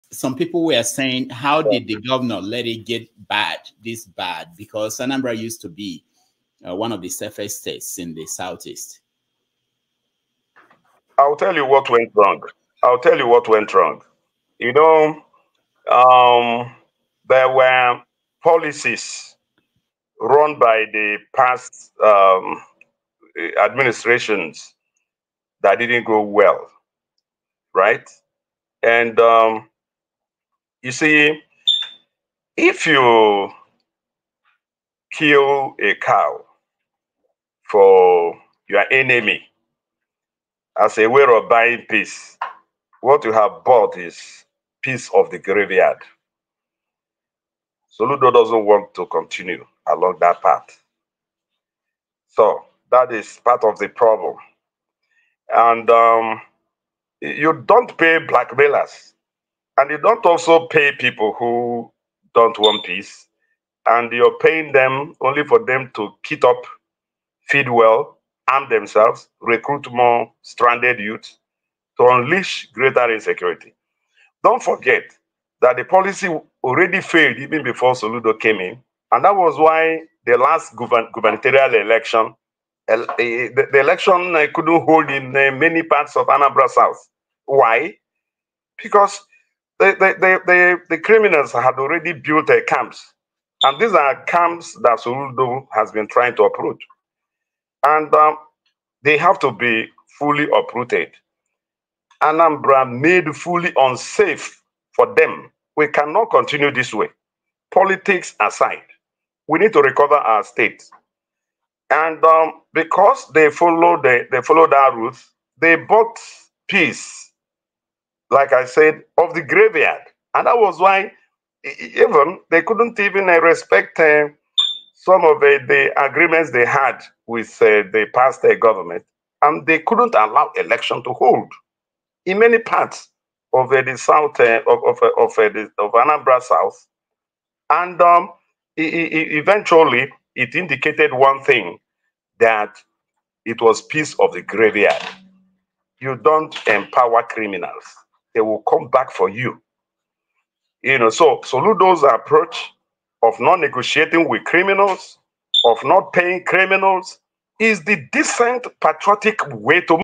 Past administrations gave money to criminals in exchange for peace in Anambra State,
The Commissioner of Information for Anambra State, Dr. Law Mefor joins 90MinutesAfrica for an interview  with Rudolf Okonkwo. He talks about Gov. Charles Soludo's Taiwan-Dubai vision, insecurity, and the upcoming 2025 election in Anambra State.

He says past administrations gave money to criminals in exchange for peace in Anambra State, something Gov. Soludo has refused to continue with.